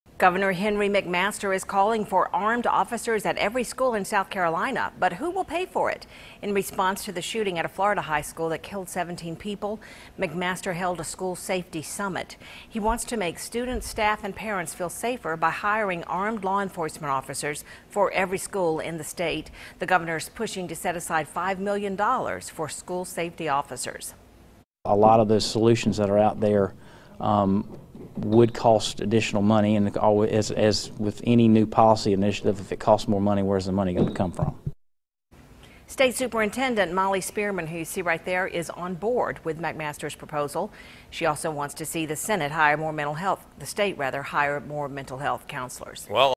Governor Henry McMaster calling for $5M to fund armed officers in schools, lawmakers question where
South Carolina Governor Henry McMaster is calling for armed officers at every school in the state, but who will pay for it?